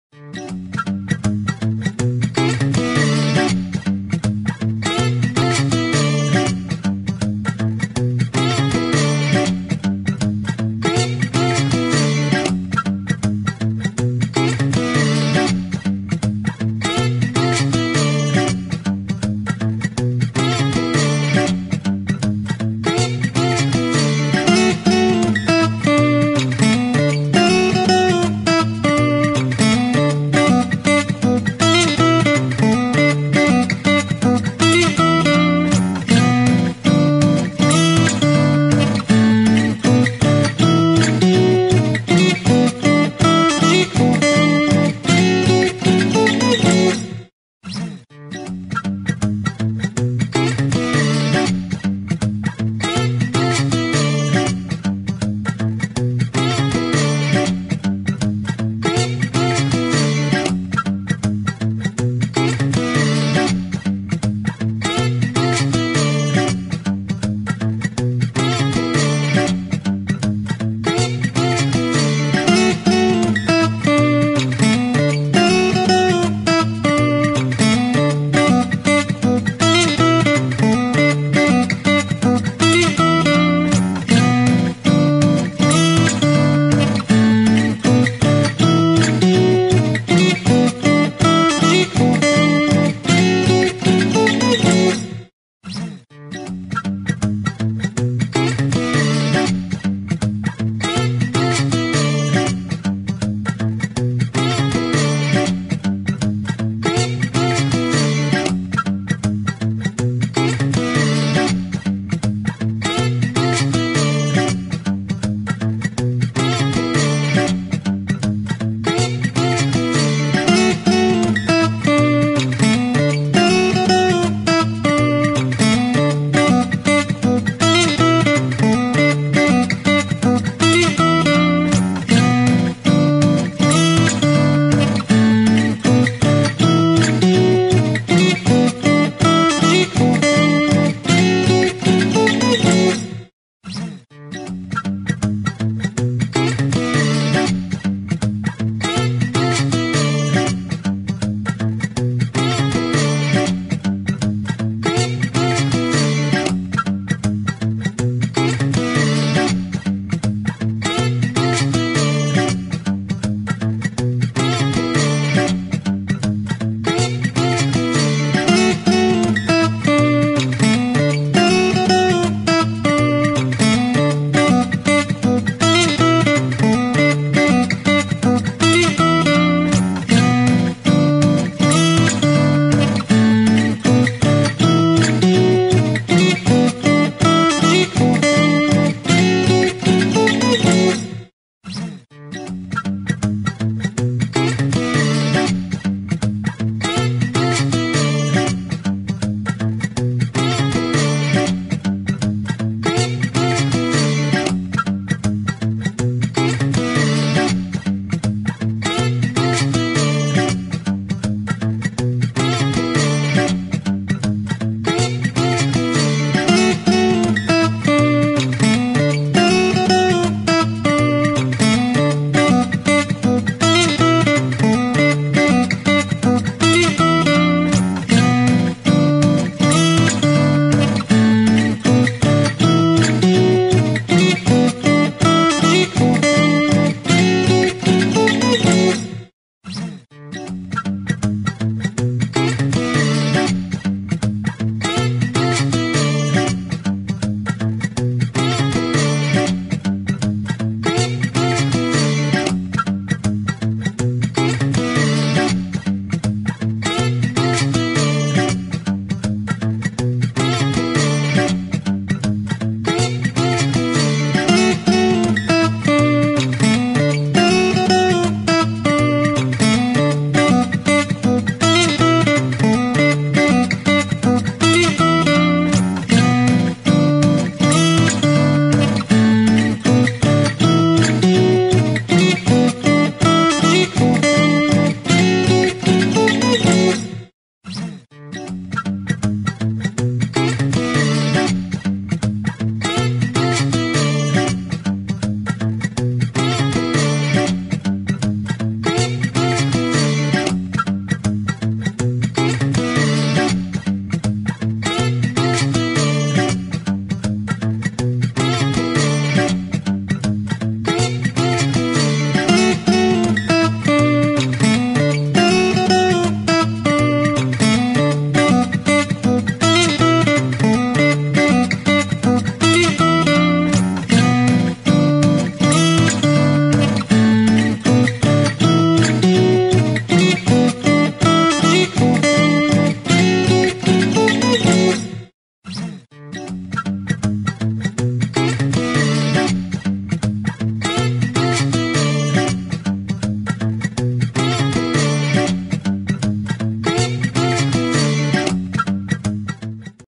Drone Flight Over Lake Anna Barberton Ohio
Quick flight with My DJI Spark
View over  Lake Anna in the Magic city of Barberton Ohio